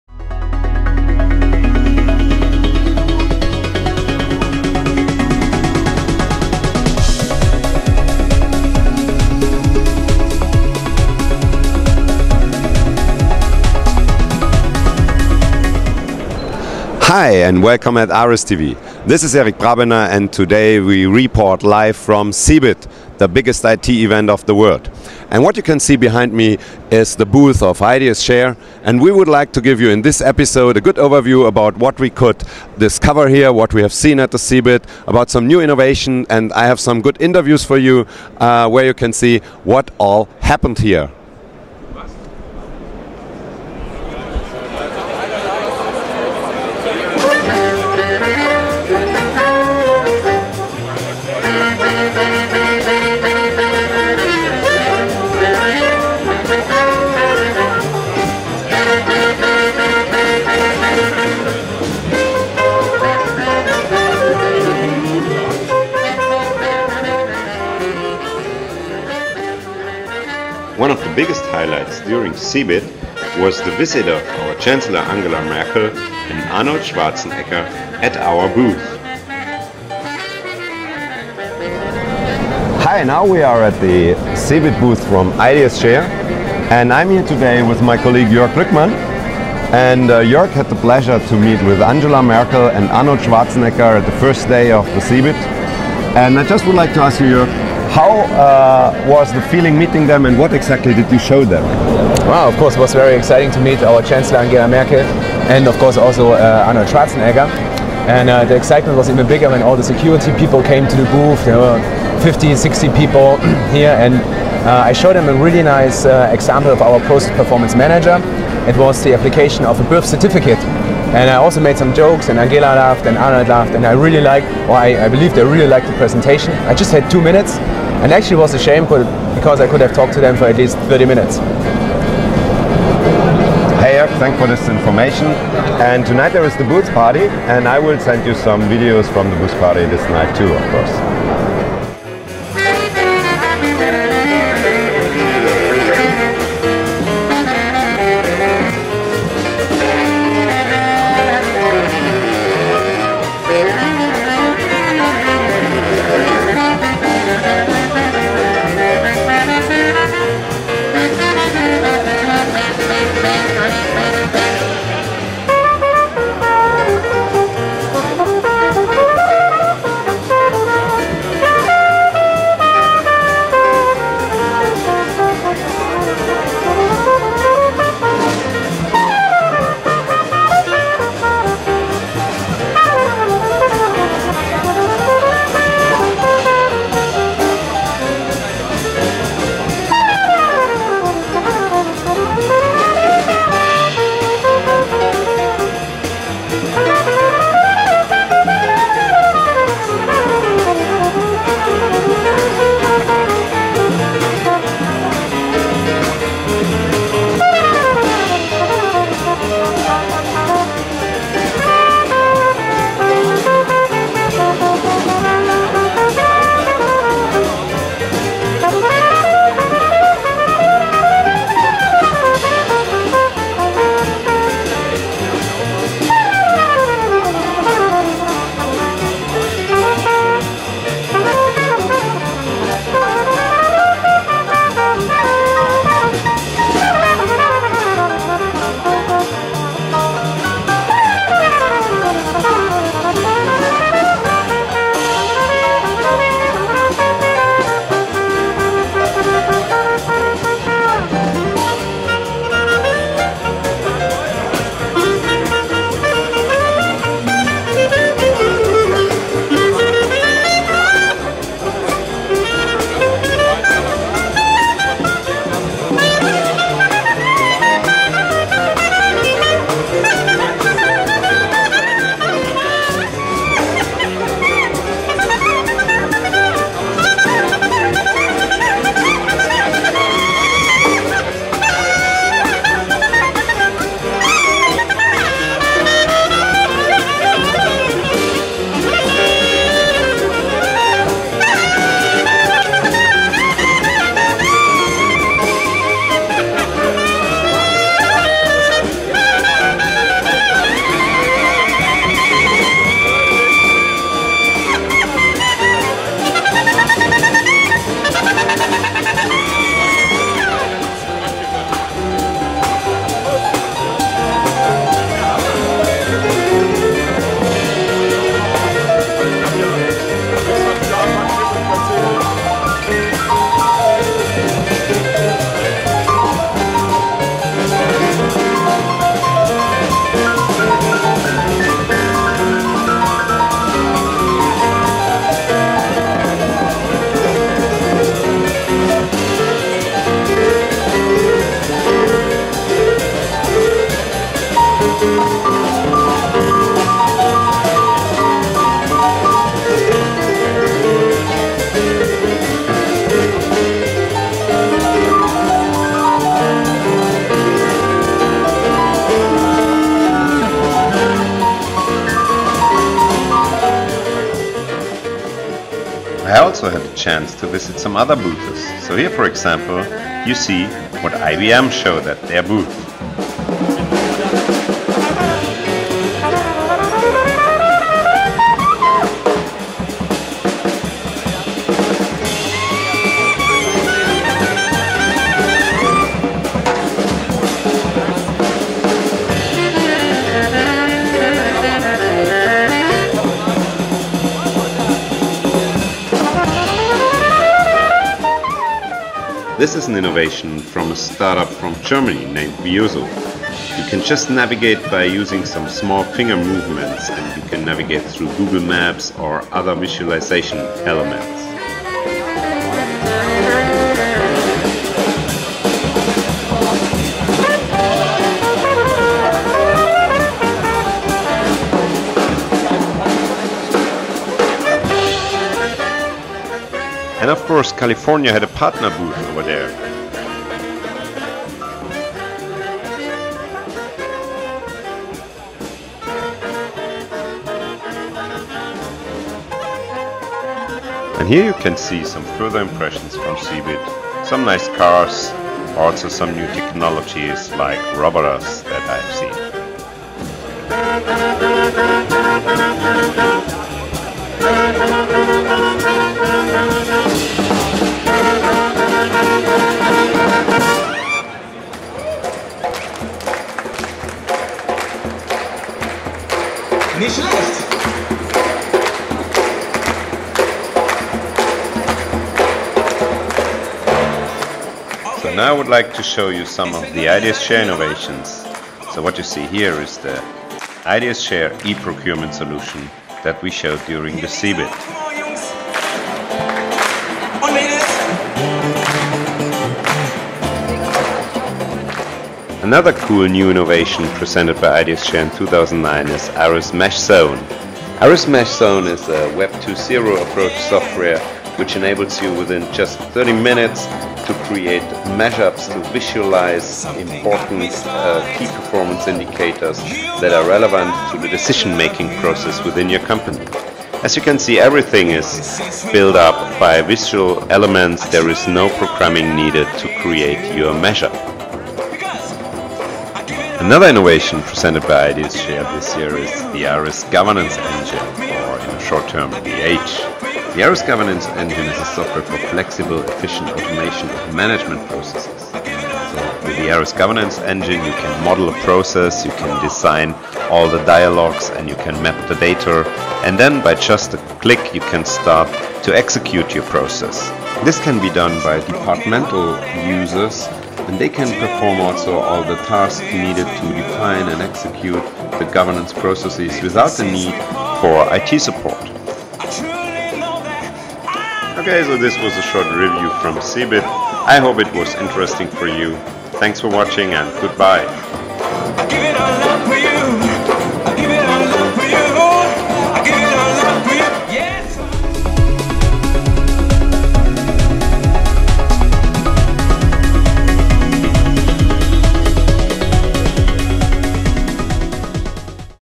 ARIS TV - 11 - CeBIT 09 Review
What are the new ARIS Innovations and what happened during CeBIT at the IDS Scheer booth.

After a longer time of silence we are providing you with a new episode of ARIS TV (episode 11) today. Okay, I have to admit: CeBIT was in March and it is really a little bit late for a direct feedback and review.
But unfortunately, there was no time to produce and publish this video earlier.

But of course, then I thought I do not want to hold back these nice impressions from our CeBIT 2009!

There was a lot of good stuff that I gathered, e.g. the visit of our Chancellor Angela Merkel and Arnold Schwarzenegger at the IDS Scheer CeBIT booth,
some nice impressions and music from our booth party, and especially Professor Scheer playing saxophone and giving a jazz session.

Of course you will also see a short overview of all the new ARIS Innovations that we already presented during CeBIT  to our prospects and customers.

So I decided to publish all this even if I am a little behind... So enjoy the video and watch it until the end to get an overview about the new cool ARIS products!